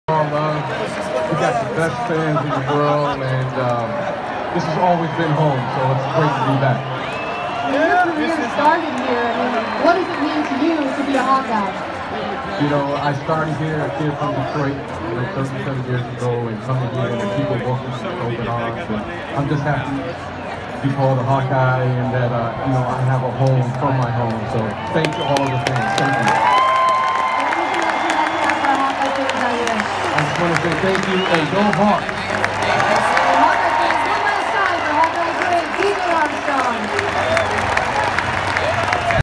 B.J. Armstrong at Kinnick Stadium
3-time Chicago Bulls dynasty champion and Los Angeles-based super agent B.J. Armstrong returned to Iowa City for homecoming.  He was on hand as the Hawkeyes hosted Purdue